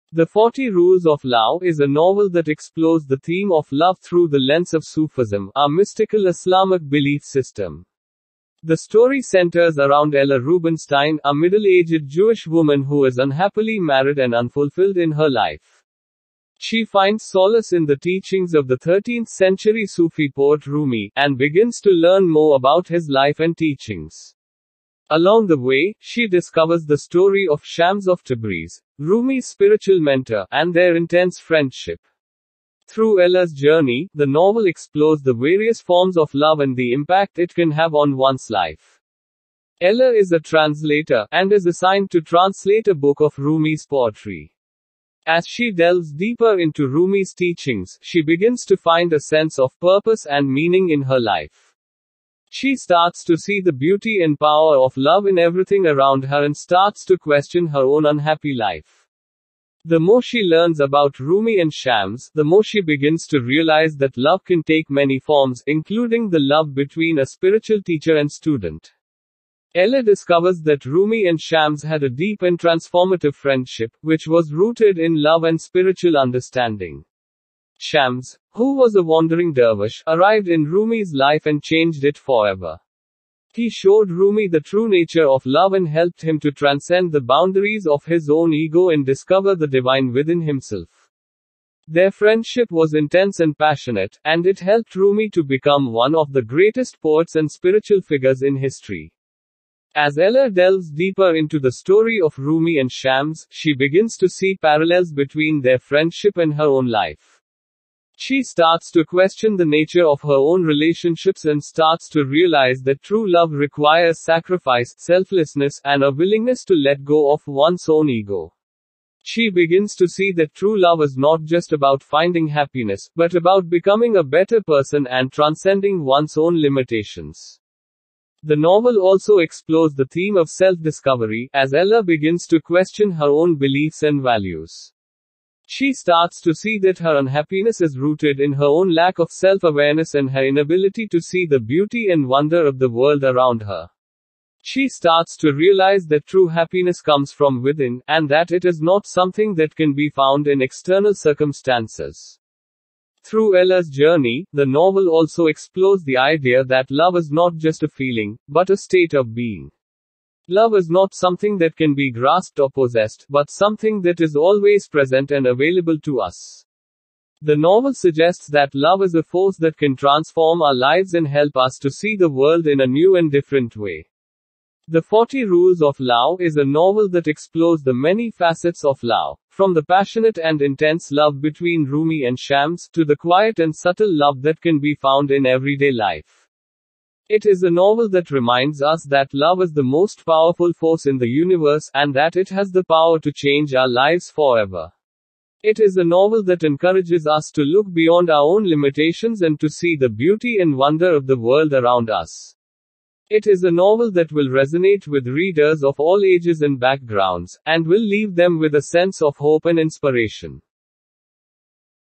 Book Summary The Forty Rules of Love by Elif Shafak
The Forty Rules of Love is a novel written by the Turkish author Elif Shafak, Her interest in writing this book was influenced by the degree she received in Gender and Women’s Studies. The book was published in March 2009. It is about Maulana Jalal-Ud-Din, known as Rumi and his companion Shams Tabrizi.